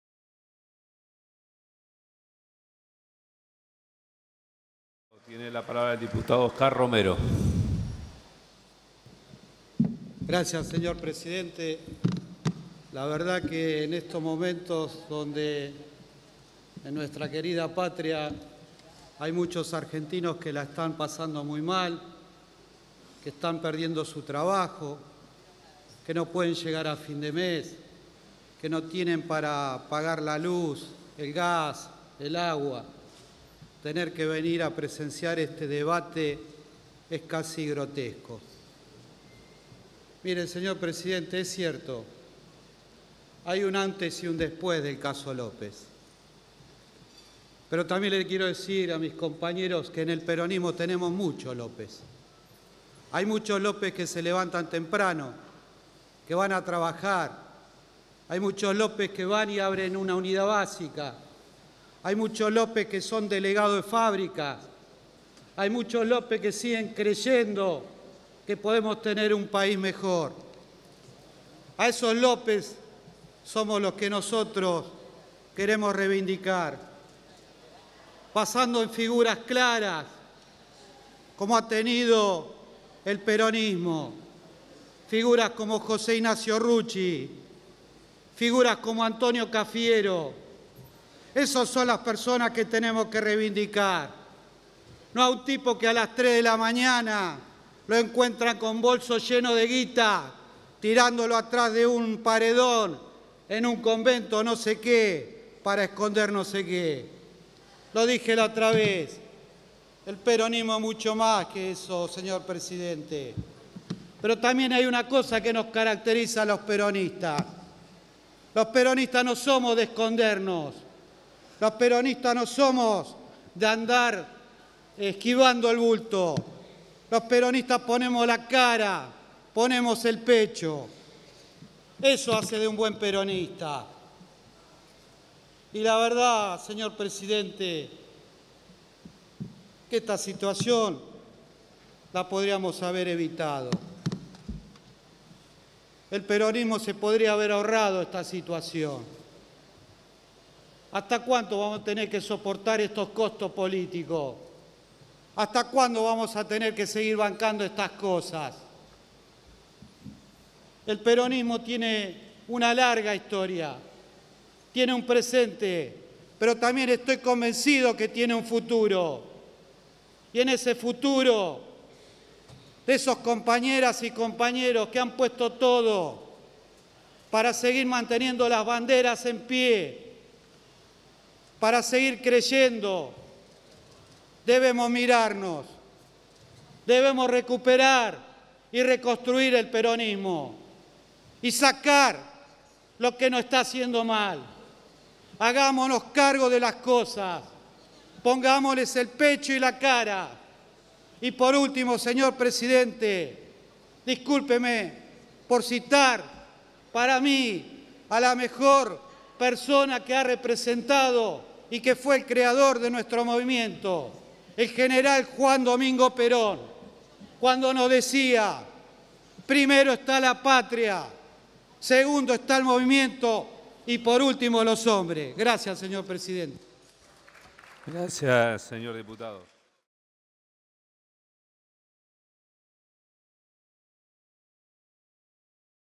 Diputado Romero Oscar Alberto - Sesión 23-06-2016
A fin de considerar el siguiente temario:

2578-D-16 DE RESOLUCIÓN.
AUTORIZAR AL JUEZ TITULAR DEL JUZGADO NACIONAL EN LO CRIMINAL Y CORRECCIONAL FEDERAL N° 9, DR. LUIS RODRIGUEZ, A ALLANAR LOS DOMICILIOS PARTICULARES Y LAS OFICINAS DEL DIPUTADO NACIONAL JULIO MIGUEL DE VIDO. ORDEN DEL DIA 228

1090-D-16 DE RESOLUCIÓN.
COMISIÓN BICAMERAL PERMANENTE DE SEGUIMIENTO Y CONTROL DEL MINISTERIO PÚBLICO DE LA NACIÓN. CONSTITUCIÓN, COMPOSICIÓN Y FUNCIONES.

1345-D-16 (y otros) DE LEY.
MODIFICACIÓN DEL CÓDIGO PENAL. INCORPORACIÓN DE ARTICULOS CREANDO LA FIGURA DEL ARREPENTIDO.

1880-D-16 DE LEY.
EXTINCIÓN DE DOMINIO SOBRE LOS BIENES PROVENIENTES DE ACTIVIDADES ILÍCITAS Y REPATRIACION DE BIENES. RÉGIMEN.

Producción Audiovisual:
Subdirección Teledifusión y Servicios Audiovisuales. Dirección Sistemas Electrónicos. Secretaría Administrativa.